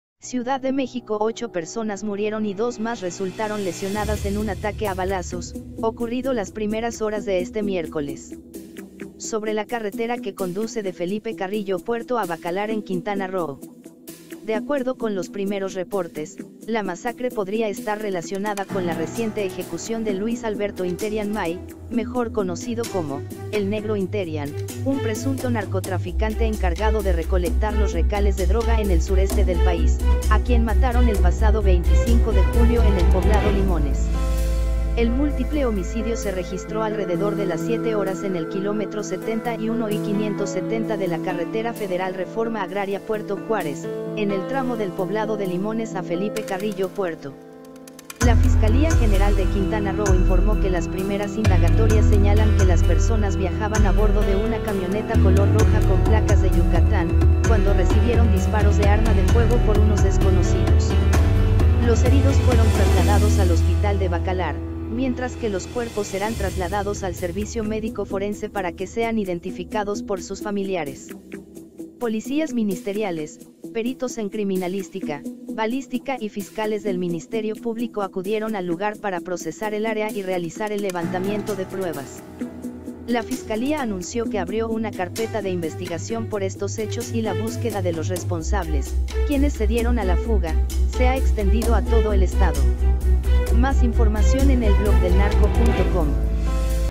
Masacre en Quintana Roo, ochos muertos y dos heridos
Ciudad de México.- Ocho personas murieron y dos más resultaron lesionadas en un ataque a balazos, ocurrido las primeras horas de este miércoles.

Sobre la carretera que conduce de Felipe Carrillo Puerto a Bacalar en Quintana Roo.  De acuerdo con los primeros reportes, la masacre podría estar relacionada con la reciente ejecución de Luis Alberto Interian May, mejor conocido como «El Negro Interian», un presunto narcotraficante encargado de recolectar los recales de droga en el sureste del País, a quien mataron el pasado 25 de julio en el poblado Limones.